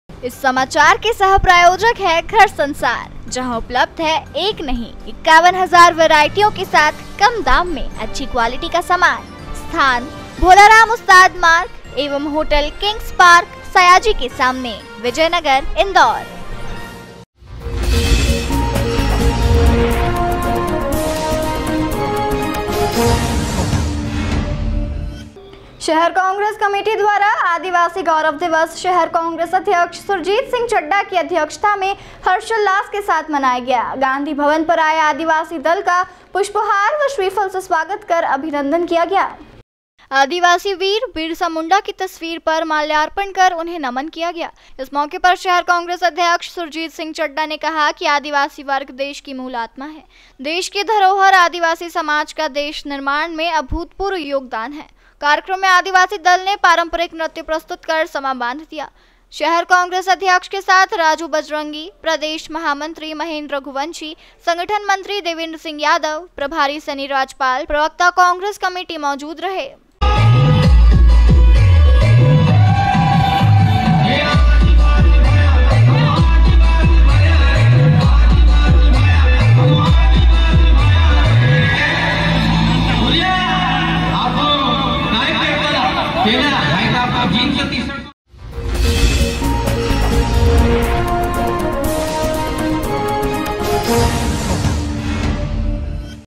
शहर कांग्रेस अध्यक्ष की अध्यक्षता में हर्षोल्लास के साथ मनाया आदिवासी गौरव दिवस
शहर कांग्रेस अध्यक्ष की अध्यक्षता में हर्षोल्लास के साथ मनाया आदिवासी गौरव दिवस#rashtrasamachar